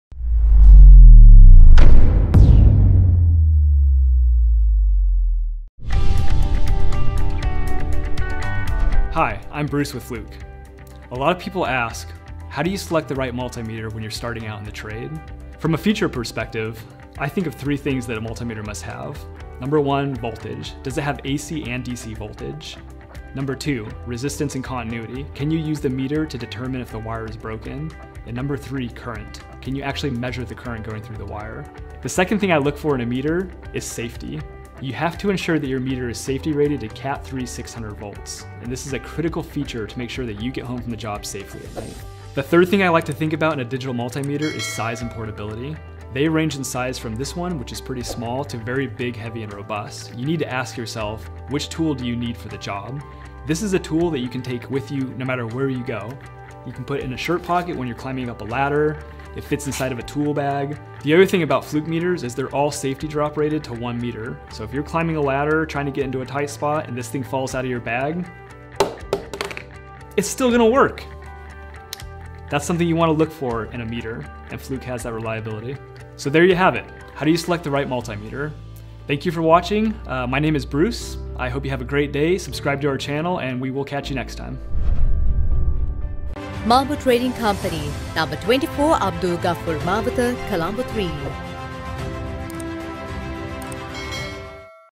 Fluke 107 Pocket Digital Multimeter
The Fluke 107 is a compact, palm size easy-to-use digital multimeter made to fit the way you work. It is the perfect first-pass troubleshooting tool; small enough to fit in a shirt pocket yet tough enough to withstand daily use, including a 1-meter drop test. For everyday applications where true-rms accuracy isn’t needed, the Fluke 107 delivers reliable measurements time after time. With a CAT III 600 V safety rating, you don’t need to compromise safety. The Fluke 107 offers a comfortable grip, weighs only 200 g, and is easy to carry.

* Fluke 107 measurement test functions
* AC and DC voltage
* AC and DC current
* Resistance
* Capacitance
* Diode
* Frequency and duty-cycle